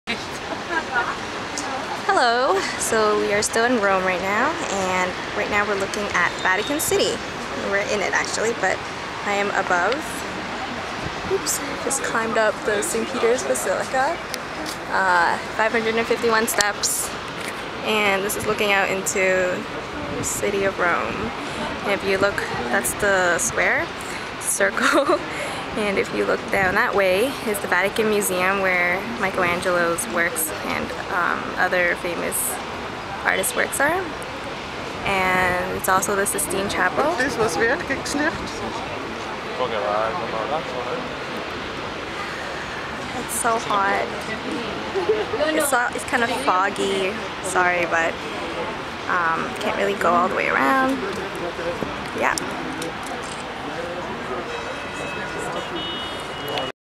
Day 30: View from St. Peter's Basilica
In Vatican City!
did you know they print their own currency? as the world's smallest country, everyone who lives in there works there and their children lose citizenship as soon as they turn 18.